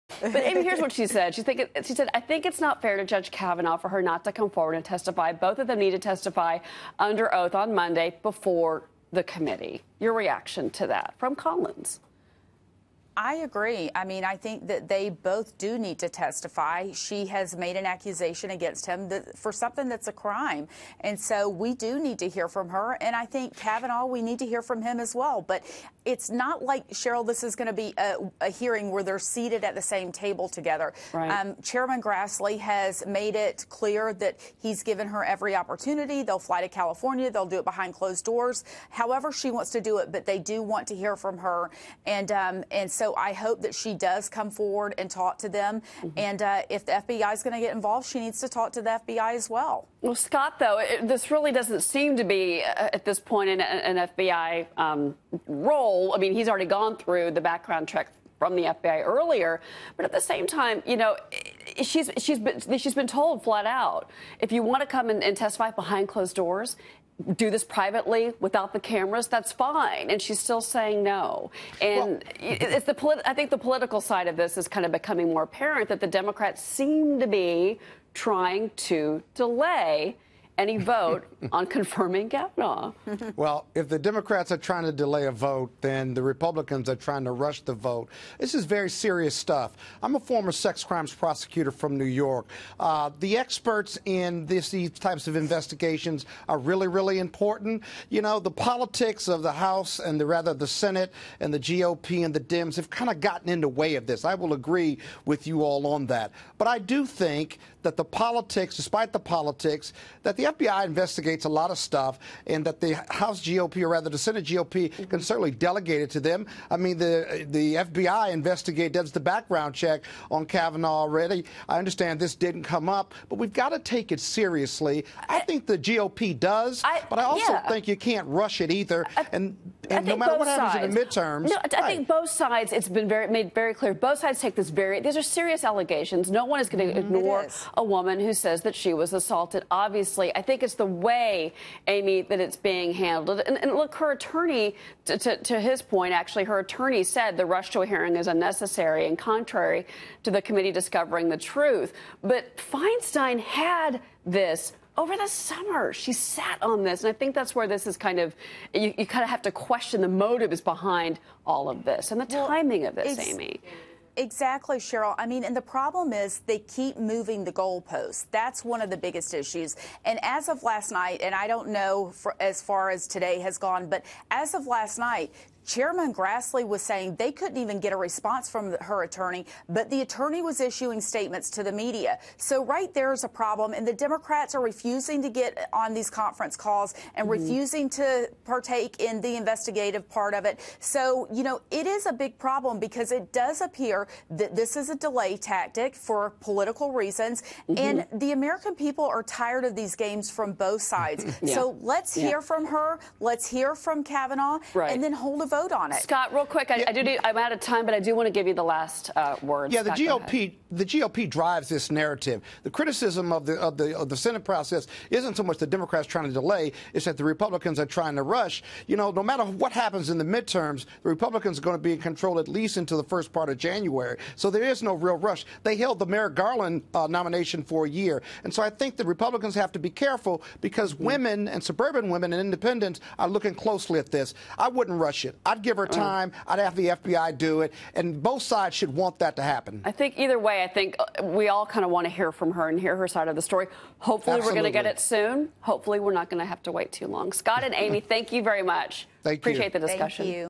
Will Kavanaugh accuser come forward and testify?
National Bar Association PAC Chairman Scott Bolden and Women for Trump co-chair Amy Kremer discuss the allegations brought against Supreme Court nominee Brett Kavanaugh by Christine Ford.